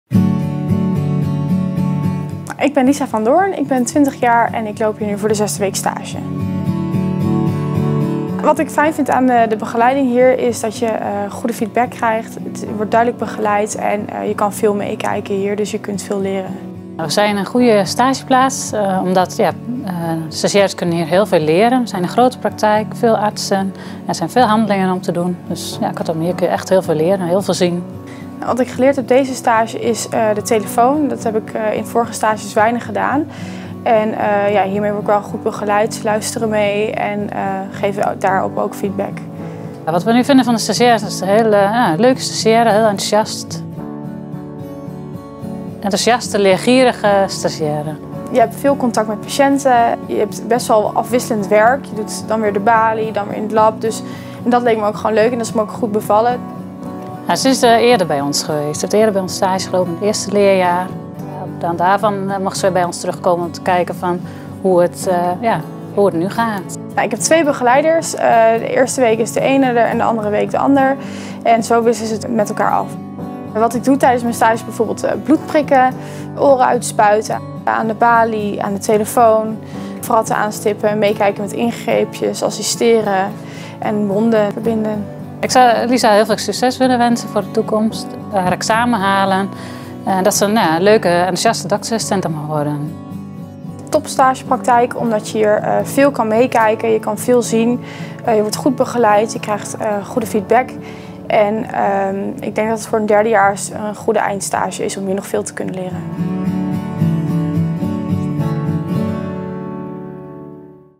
Deltion beoordeelt stageplaatsen Doktersassistenten 2016-Dalfsen
Deltion bepaalt in samenwerking met Medrie welke stageplaats voor doktersassistenten dit jaar het beste is.Beschrijving